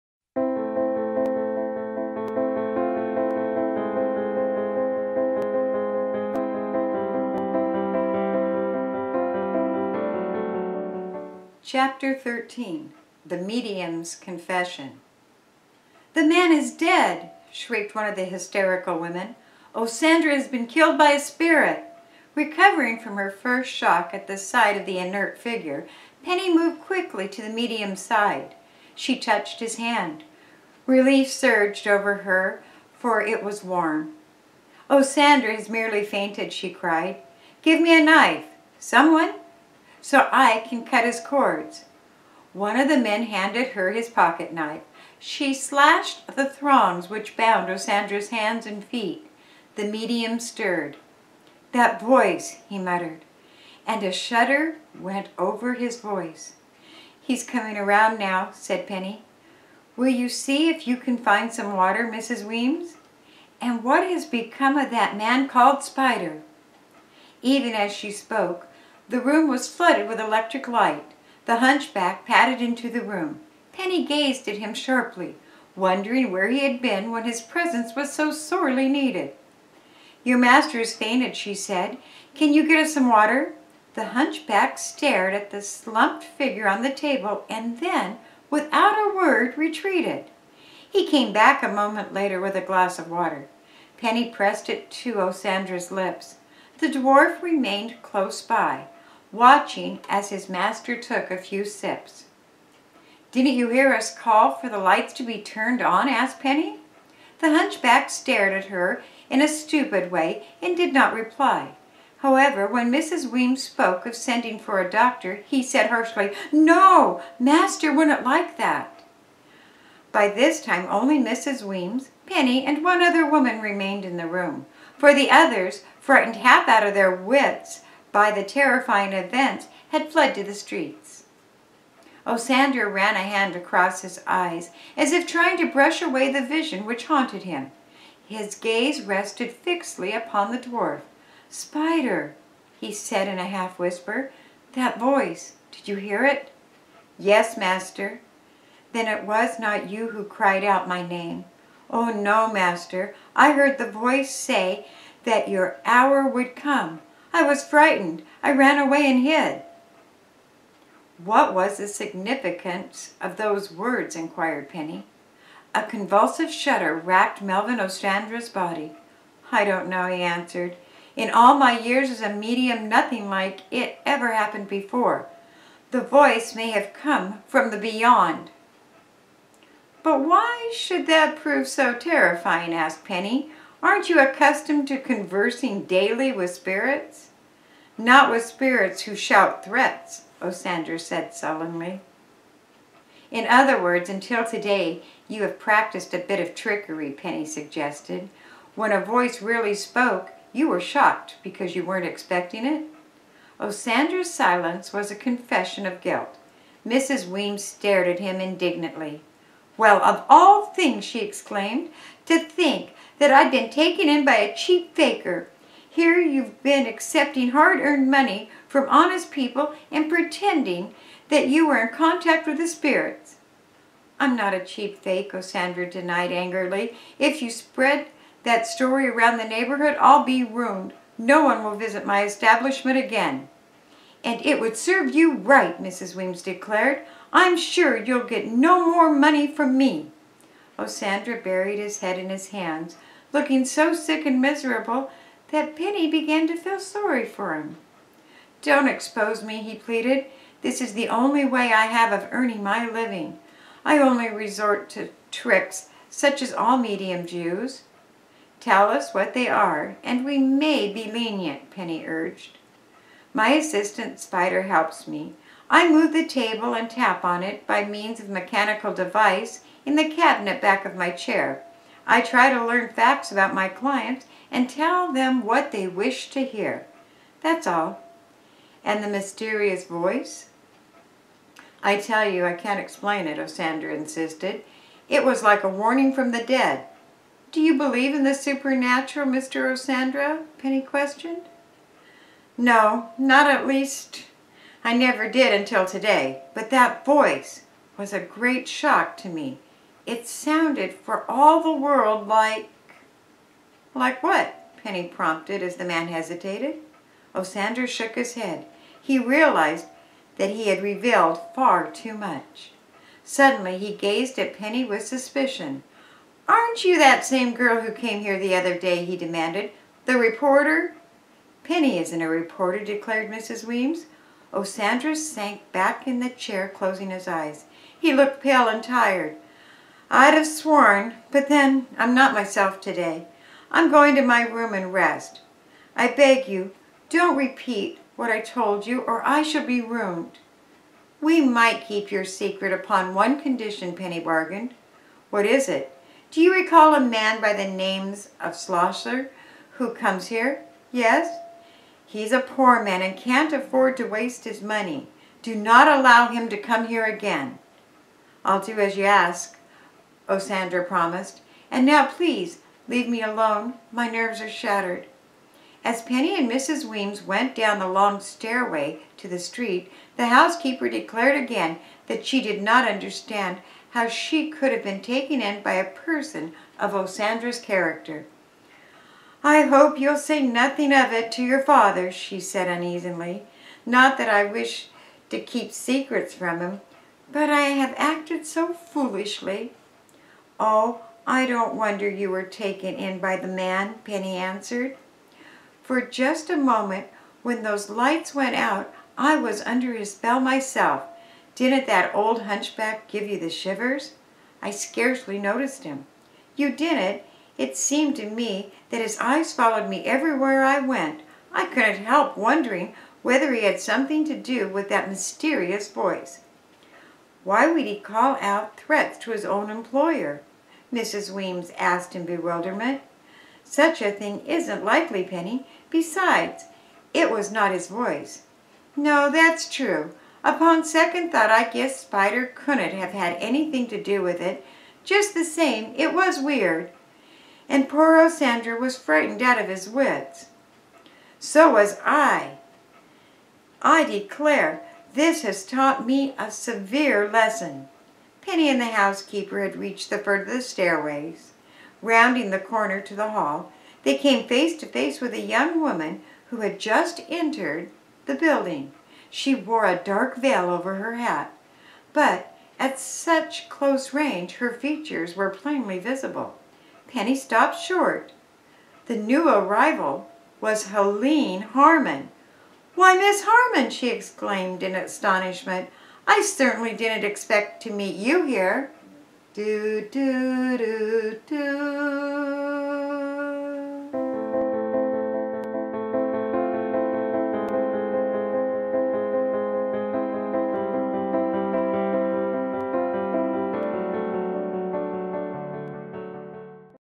Penny Parker Mystery: Tale of the Witch Doll - Chapter 13
Grandmama Presents:

Penny Parker Mystery Stories
By Mildred A. Wirt

Publisher: Cupples and Leon Co, New York

Authors Note:
Penny Parker starred in a series of 17 books written by Mildred A. Wirt Benson and published from 1939 through 1947.

Penny was a high school sleuth who also occasionally moonlighted as a reporter for her father's newspaper.

Benson favored Penny Parker over all the other books she wrote, including Nancy Drew.   Her obituary quoted her as saying, "I always thought Penny Parker was a better Nancy Drew than Nancy is." Mrs. Benson said in 1993.

Mildred Augustine Wirt Benson (July 10, 1905 – May 28, 2002) was an American journalist and author of children's books. She is best remembered for writing some of the earliest Nancy Drew mysteries and for creating the detective's adventurous personality. Benson wrote under the Stratemeyer Syndicate pen name, Carolyn Keene, from 1929 to 1947 and contributed to 23 of the first 30 originally published Nancy Drew mysteries which became bestsellers.

Read by  Grandmama 2018